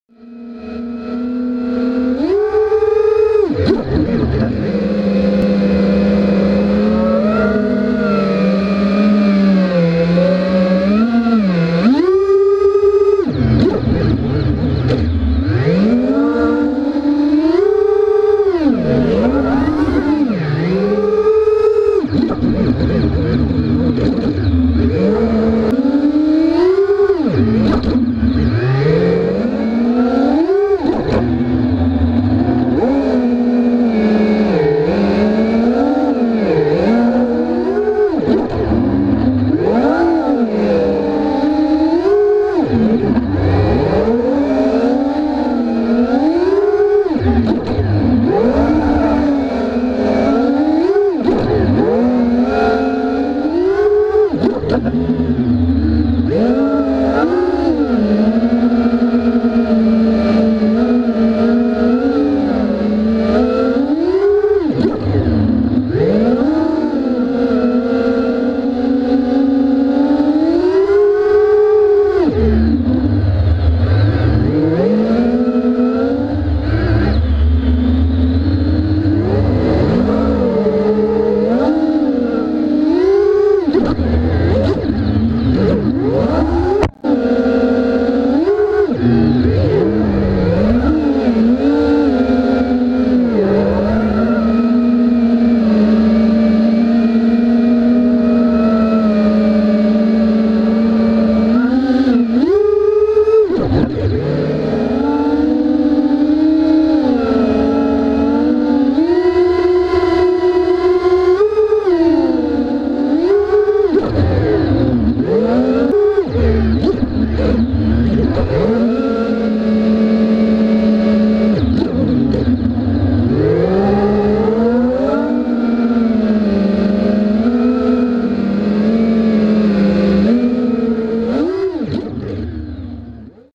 Dalrc XR215 V2 - Training on Betaflight 2.8.1
Configuration :
- Frame : Dalrc XR215 V2
- FC : Naze32 Rev6 C - Betaflight 2.8.1 (stock PID)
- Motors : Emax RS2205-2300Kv
- Props : Gemfan PC 5x4x3
- ESC : Litllebee 30A (Fw 14.6)
- vTx : Skyzone TS5823S 200Mw
- Antenna : Aomway RHCP
- FPV Camera : Runcam Swift
- Camera : GoPro Hero 3+ BE
- Lipo : Tattu 4S 1550mah 45-90c